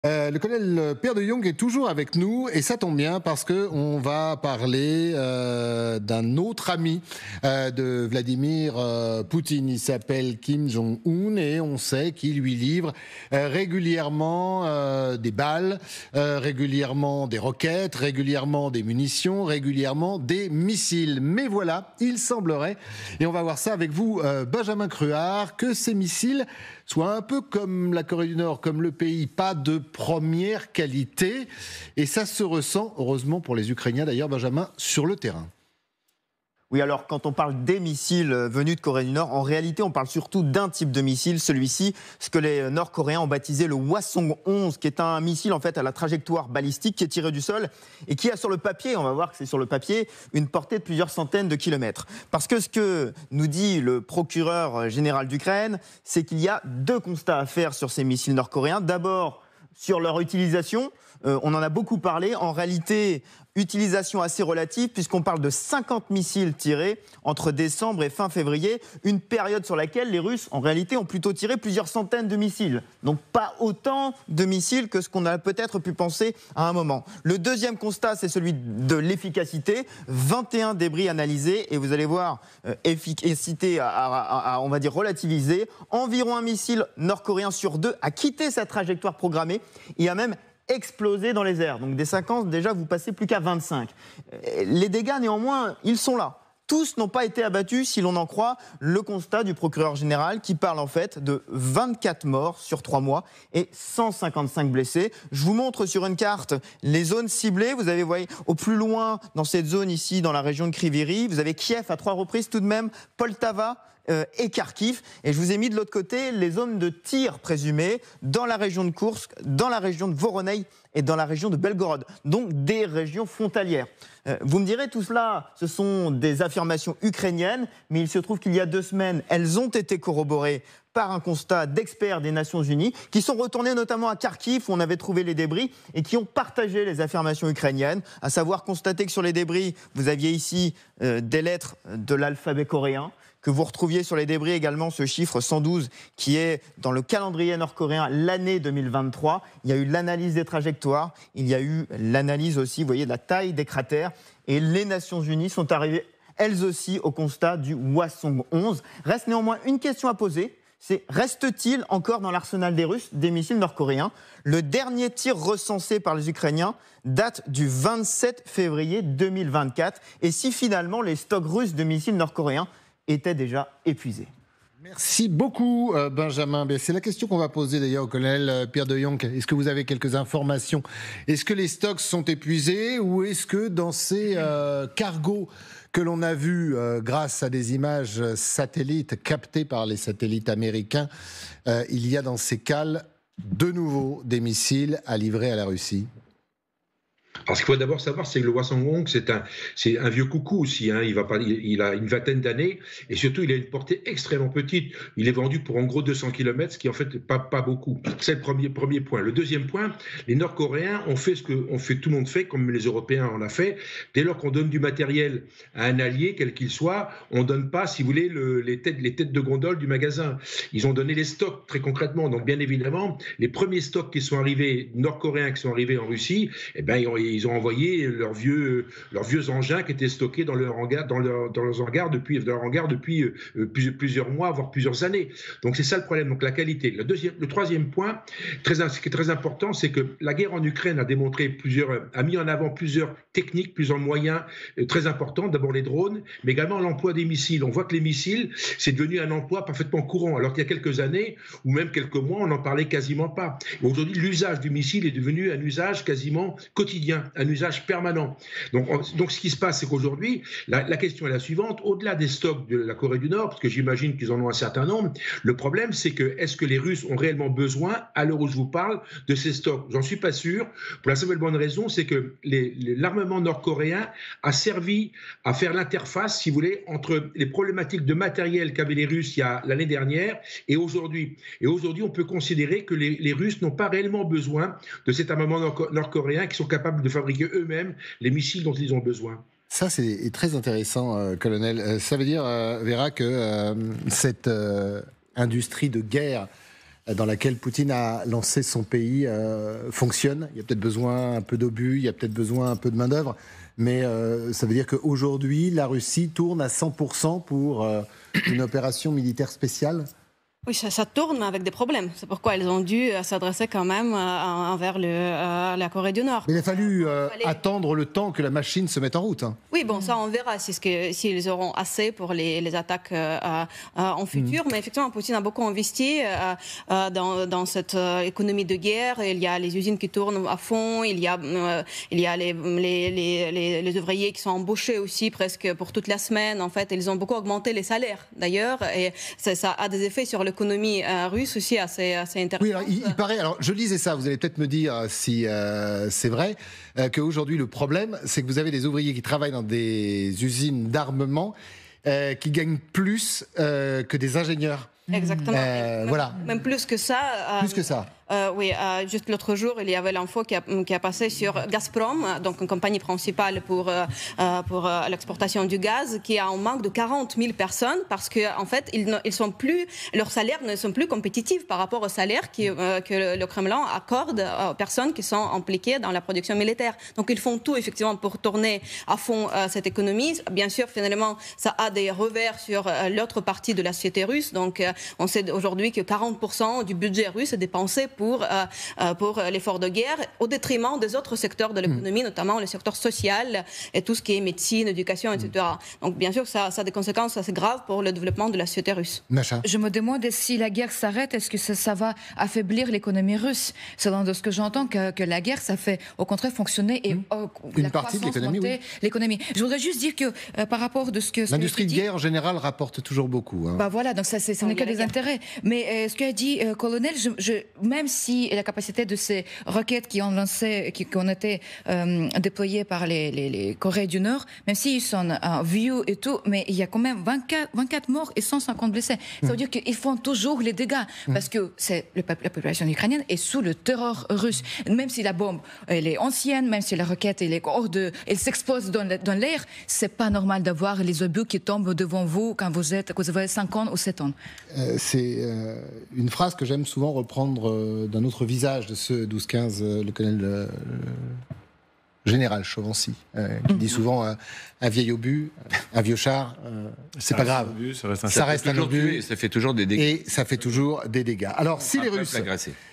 Corée du Nord : les armes livrées à la Russie dépassées
La Corée du Nord livre des armes et munitions à la Russie. Mais il semblerait que les missiles Hwasong-11 ne soient pas de première qualité et ça se ressentirait sur le terrain.